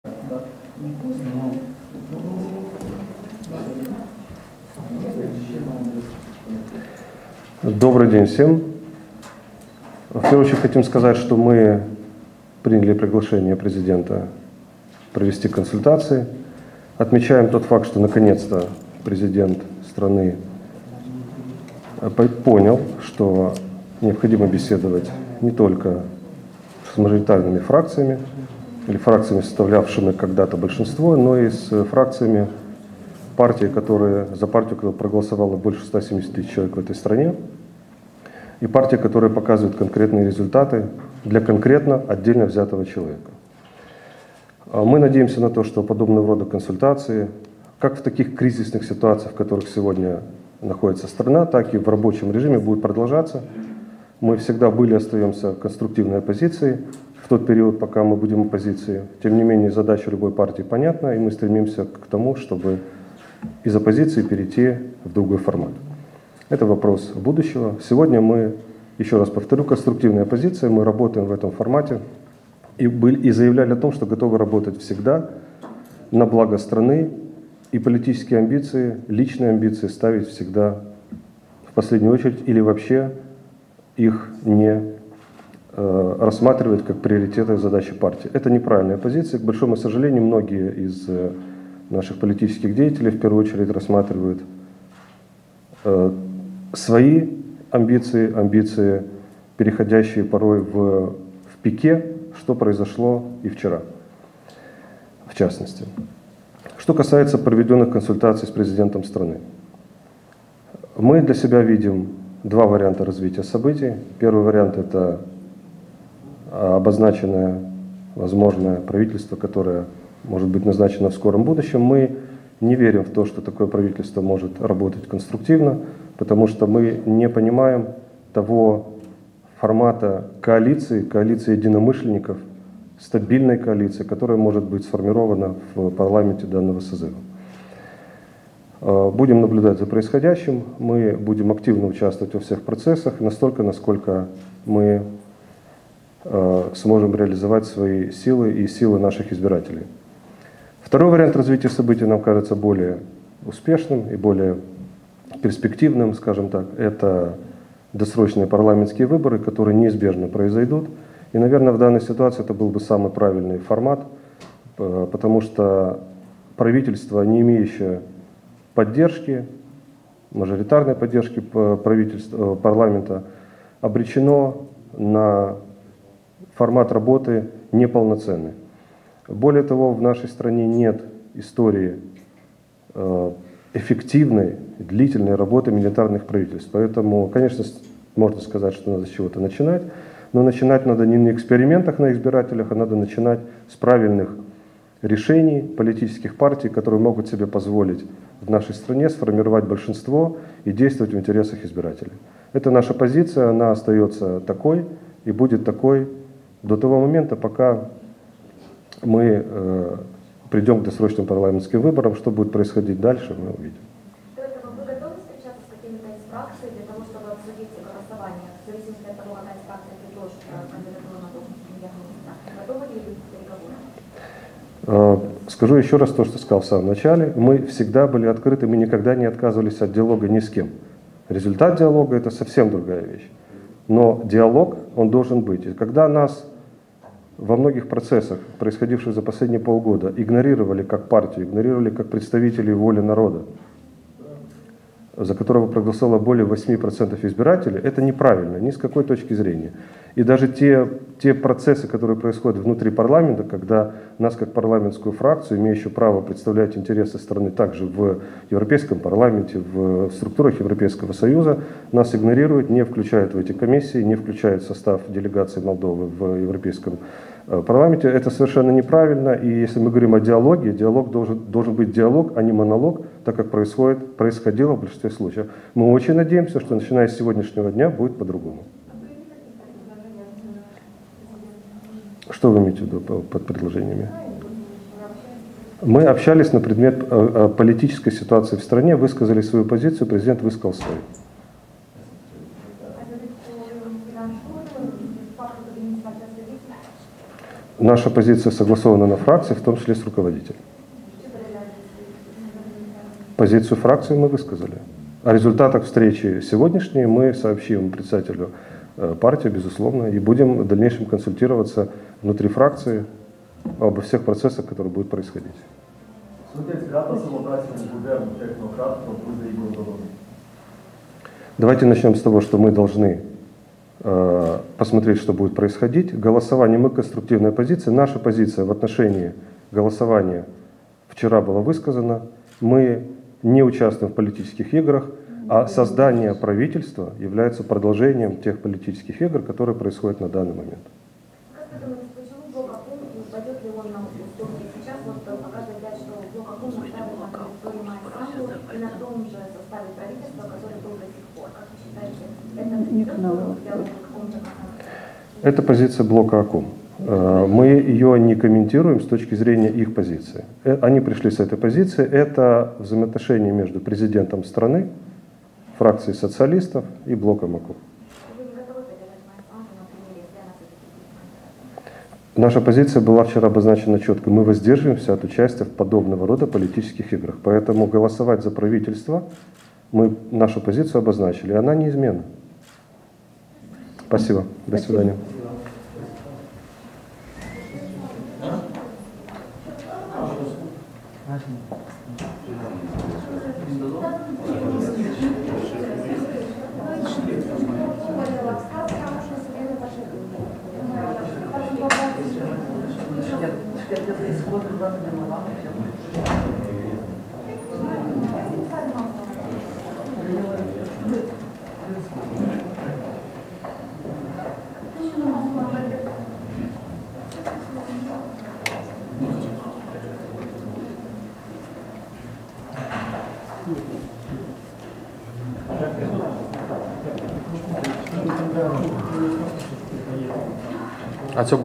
Declarația Partidului ȘOR după întrevederea cu Președintele Republicii Moldova, Igor Dodon
Declarația reprezentanților Partidului ȘOR după întrevederea cu Președintele Republicii Moldova, Igor Dodon